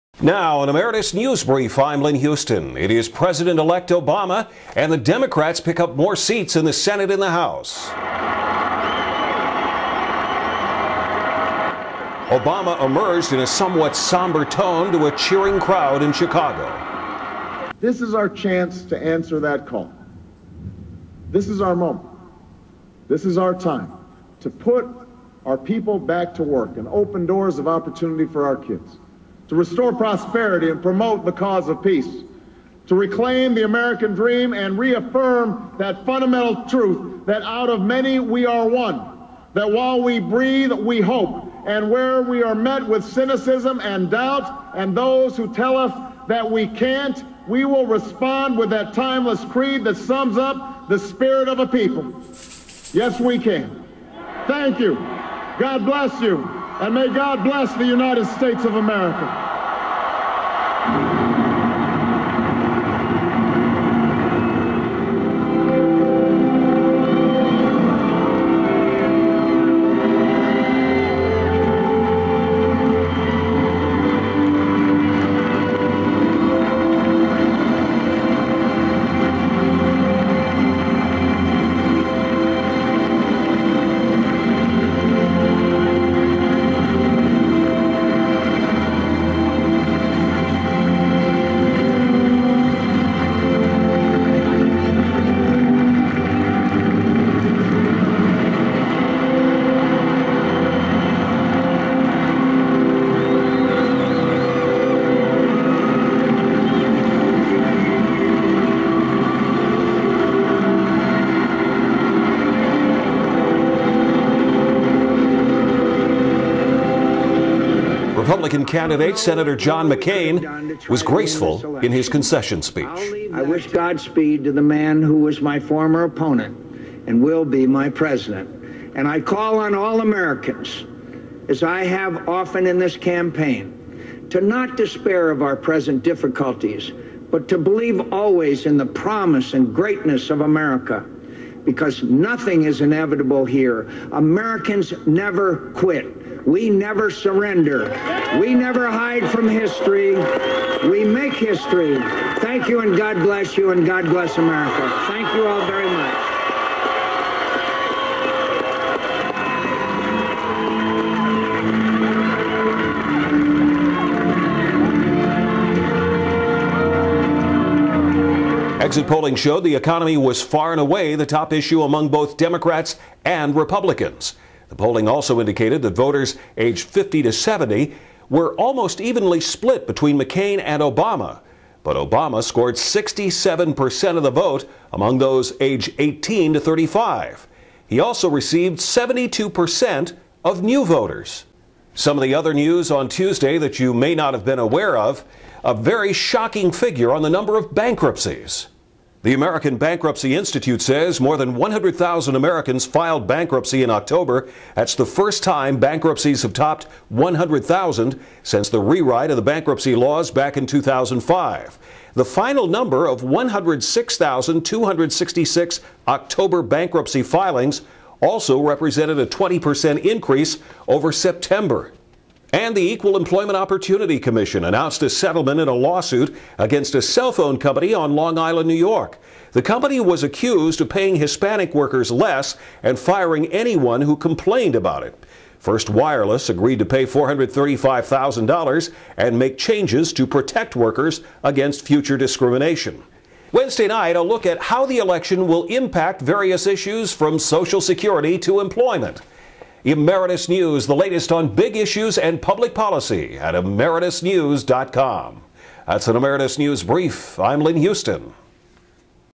Emeritus Newsbrief November 4 2008
Lynn Huston anchors Emeritus Newsbrief
Obama wins.
Obama and McCain speeches.
Bankruptcies reach new high.
Pay discrimination case settled.